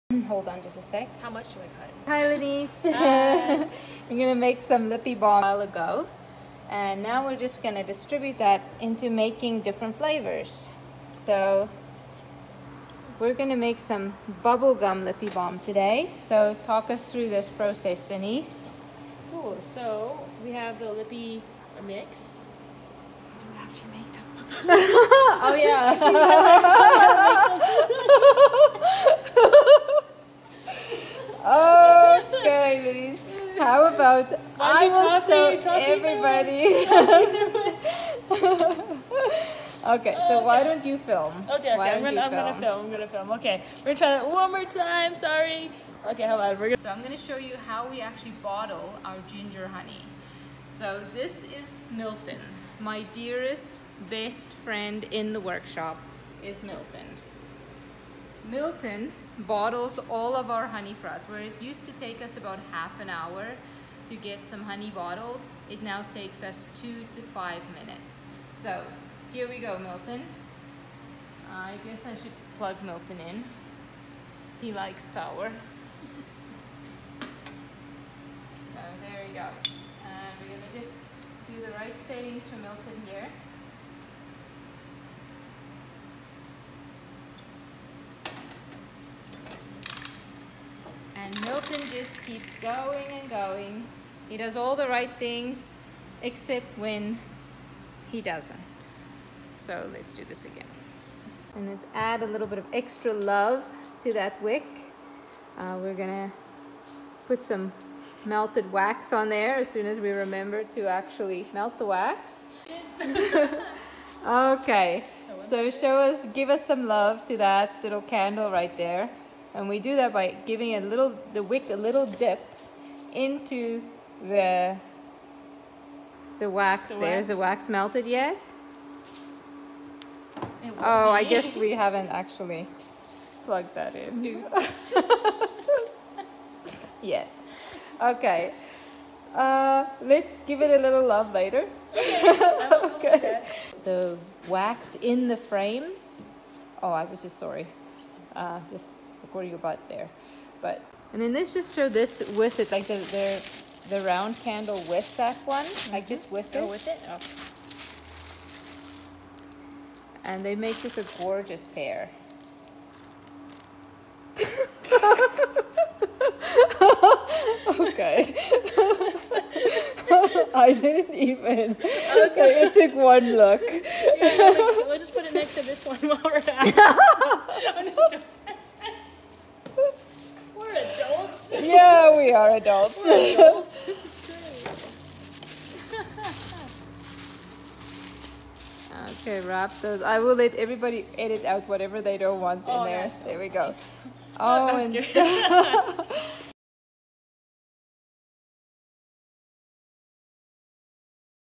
Forever Bee Facebook Live Event - 8 Bloopers
Blooper reel from Forever Bee Facebook live event, hosted March 28, 2020.

Forever Bee is an Alberta, Canada based apiary serving the local area and beyond.